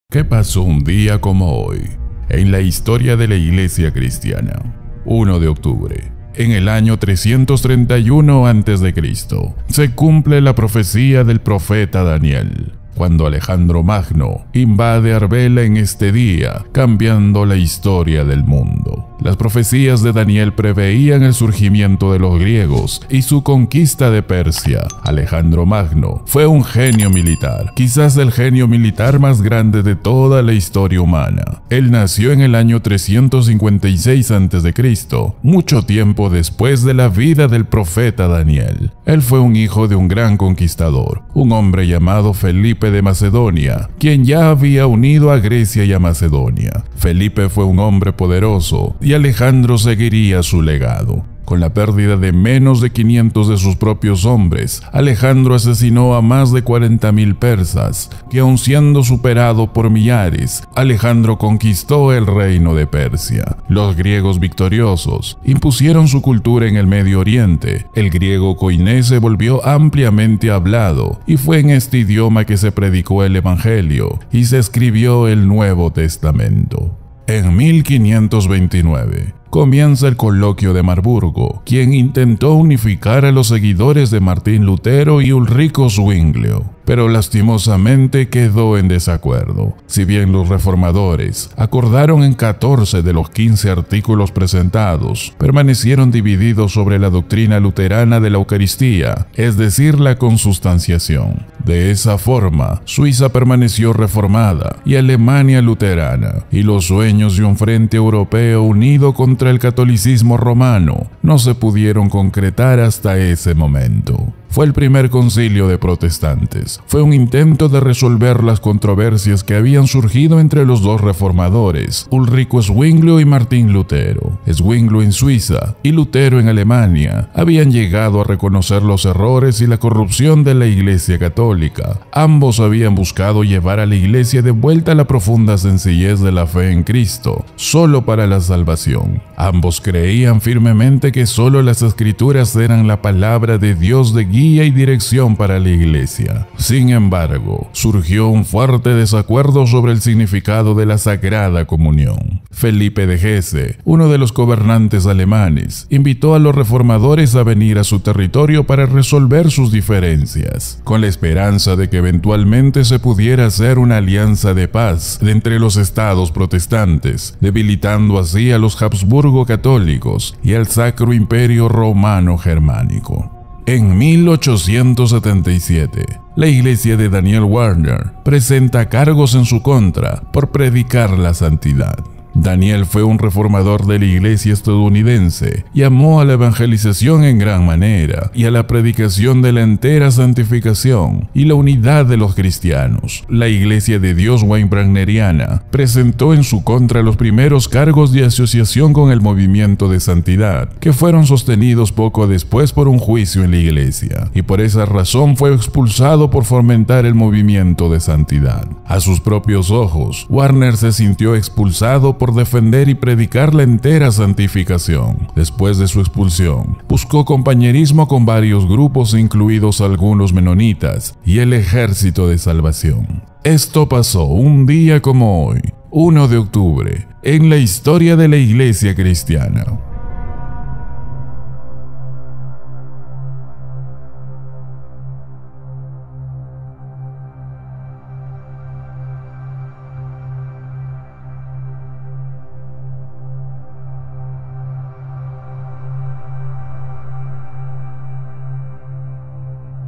Se cumple PROFECÍA bíblica del profeta DANIEL ✝️📕 UN DÍA COMO HOY en LA HISTORIA CRISTIANA
Un día como hoy en la Historia Cristiana Bíblica ¿Qué paso un día como hoy en la historia de la Iglesia Cristiana evangélica protestante? -  30 de Septiembre. Esto pasó un día como hoy en la historia de la Iglesia Cristiana Evangélica

Se cumple la profecía bíblica del profeta Daniel. Mira el video

- Mensaje extraído de John Macarthur,  Grace Ministries and Grace Community Church
ℹ️ Para más información sobre nosotros, visite nuestra página web